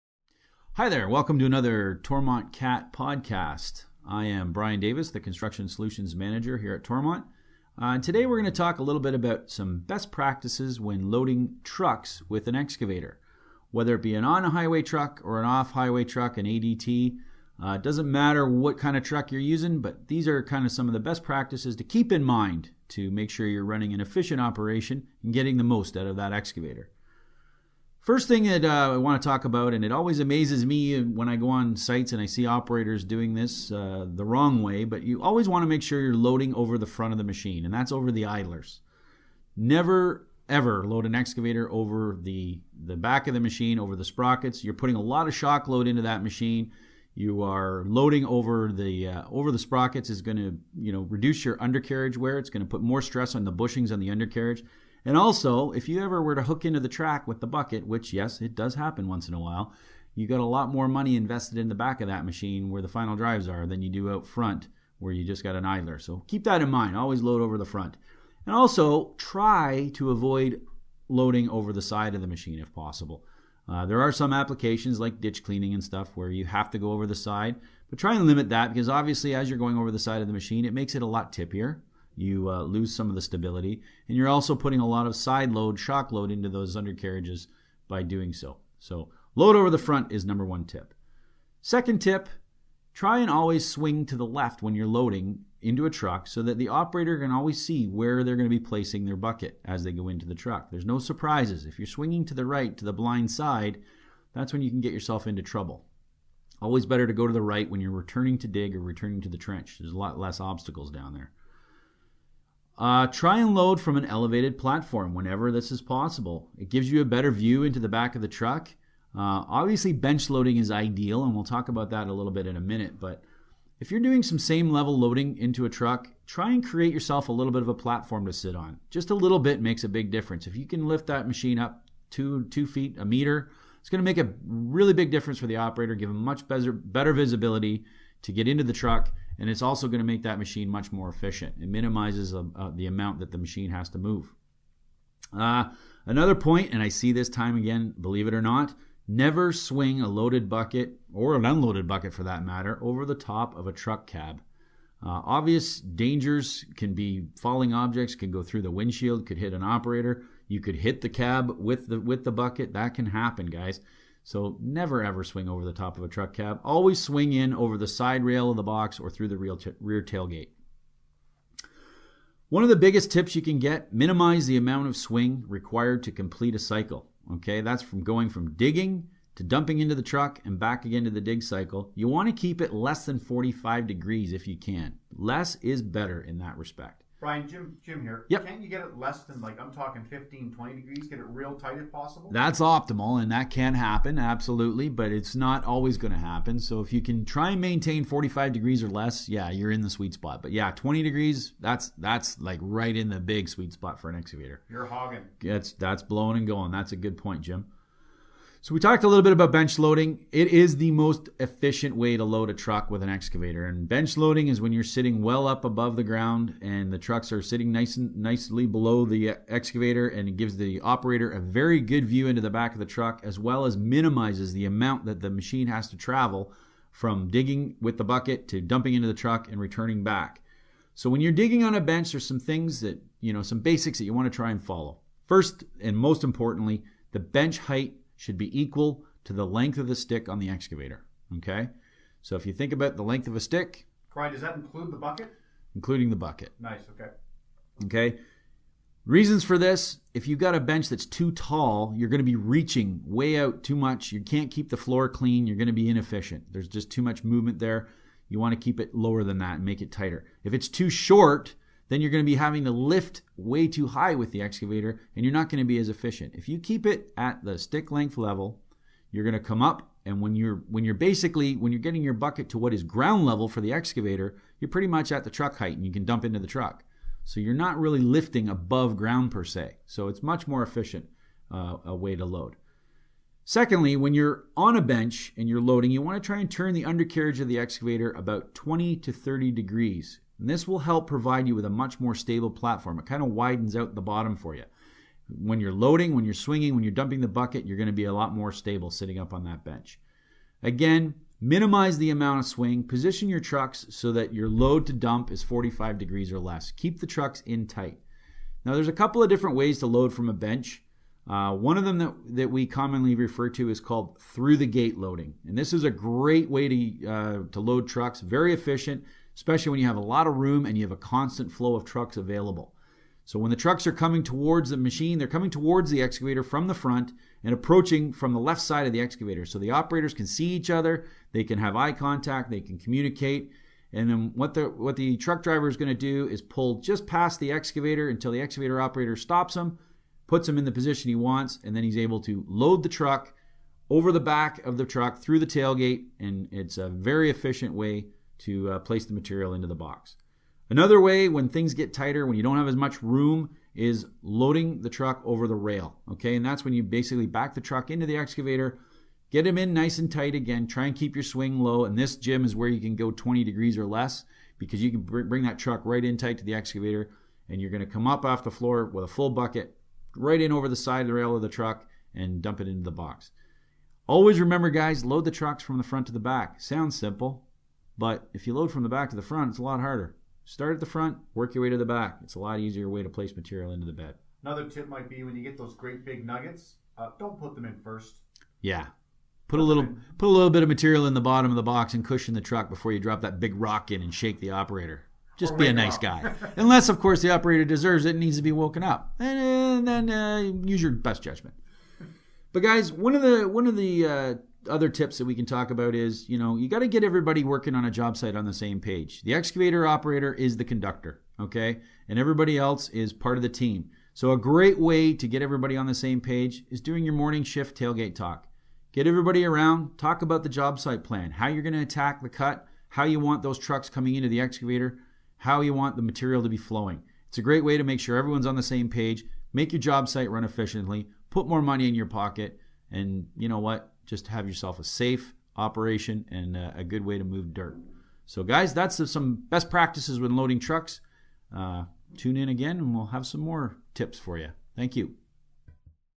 Toromont Cat In The Iron Podcast # 8 Excavator Best Practices
In this Toromont Podcast you’ll hear from an excavator loading efficiency expert. Brian Davis specializes in visiting sites to identify issues and improve processes – he’s seen excavators used in great and less than great ways. He’s going to teach you how to get the most of loading trucks with your excavator so your machine will last longer, create a safe loading environment and making more money.

You will hear:

Why loading over the front makes a HUGE difference
Does swinging to the left or to the right make a difference?
What is bench loading and why is it important
Safely and efficiently loading a Truck